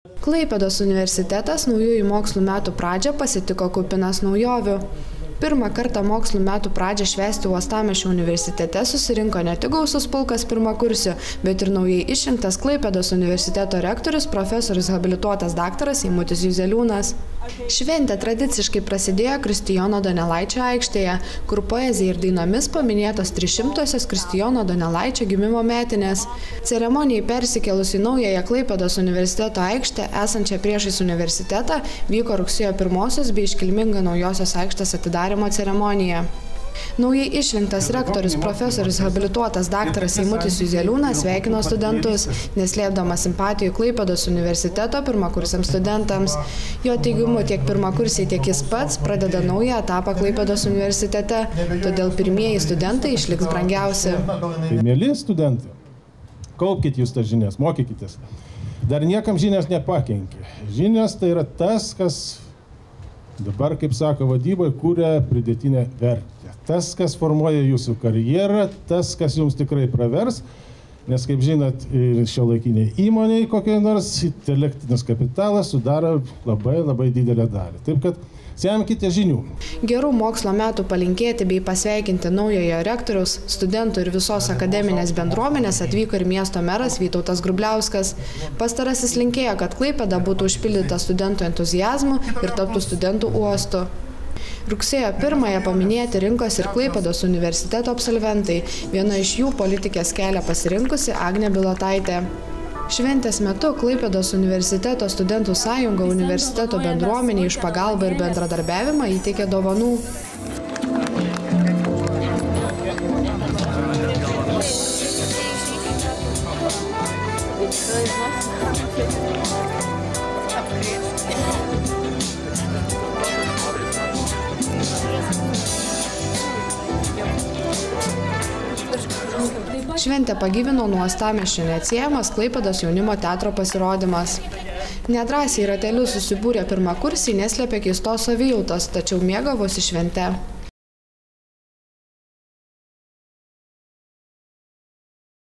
Rugsėjo pirmoji Klaipėdos universitete
Rugsėjo pirmoji kiekvienam jaunam asmeniui turi šiokią tokią reikšmę – vieniems tai naujos pradžios atskaitos taškas, dar vieni metai mokymo įstaigoje, o kitiems – naųjų darbų pradžia. Laidos universitetai.lt komanda domėjosi, šios šventės reikšme uostamiesčio universitete.